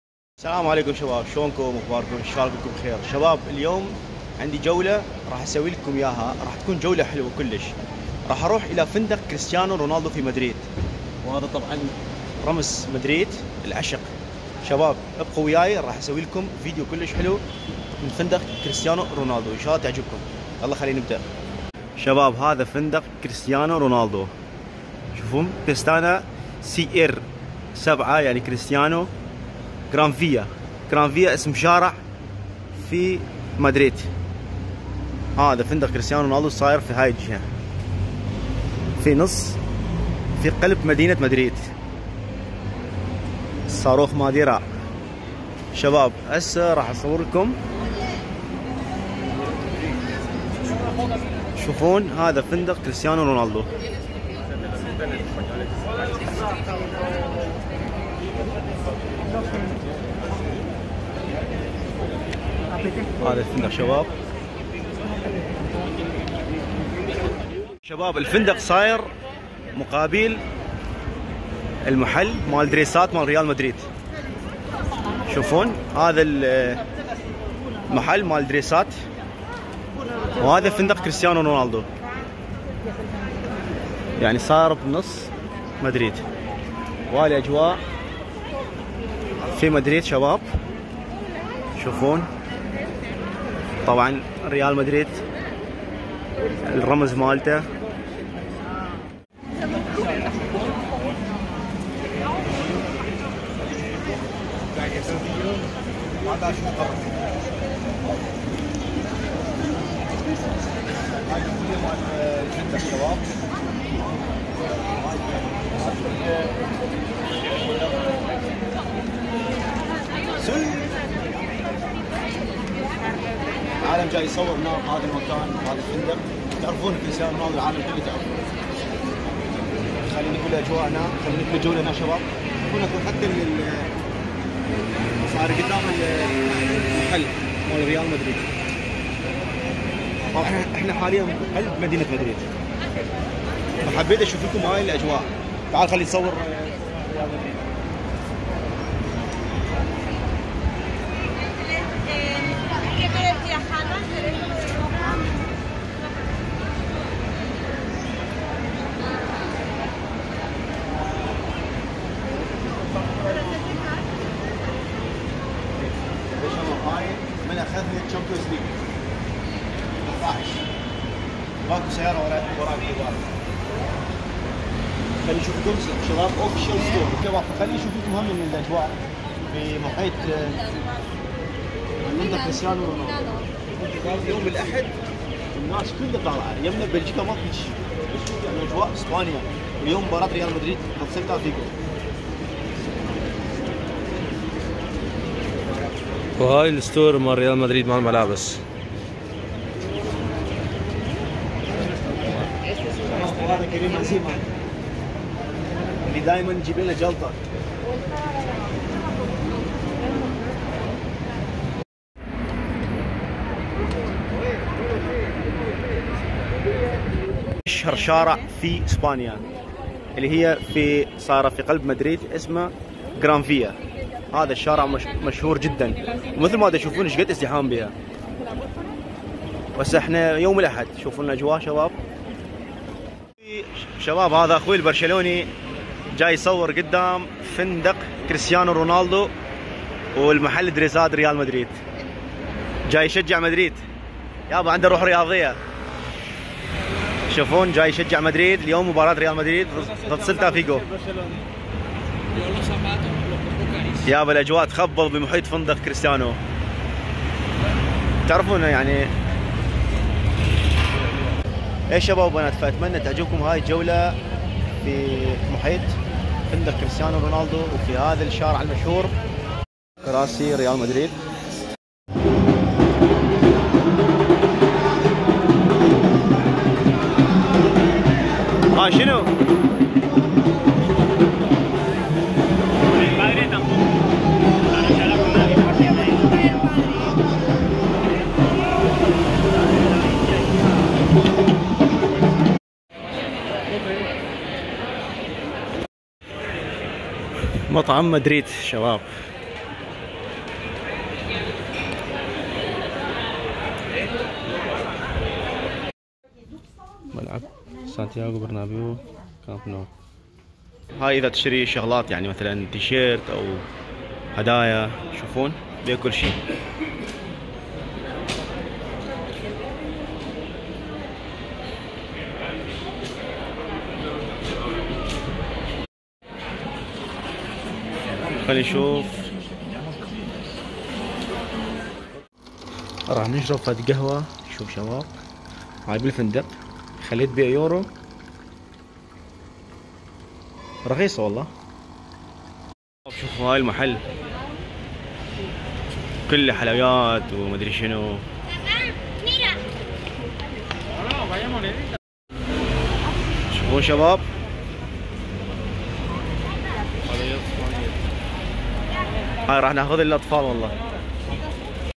فندق كرستيانو رونالدو في قلب مدريد 😲🇪🇸| جولة في اشهر شارع في مدريد. زرت محل ريال مدريد
مرحبا حبايبي الغاليين. سويت هذا الفيديو في قلب عاصمة اسبانيا، مدريد. رحت إلى فندق كرستيانو رونالدو و صورت في اشهر شارع في اسبانيا. الأجواء  خرافية في شارع Gran via في مركز مدينة مدريد . بصراحه ما صدقت تكون هيجي الأجواء في مدريد و محيط فندق كرستيانو رونالدو و الاستور ريال مدريد.

اتمنى تستمتعون بالاجواء و تعجبكم الفيديوات. تعرفون القناة متنوع و احب انشر على كل شيء مو بس لبهج و القوانين الأوربية.  مرات احب انشر فيديوات متنوعه و اكيد على ذوق الجميع.

حسابي الانستاكرام: Stevaniraki

حسابي الفيسبوك: Stevan Najim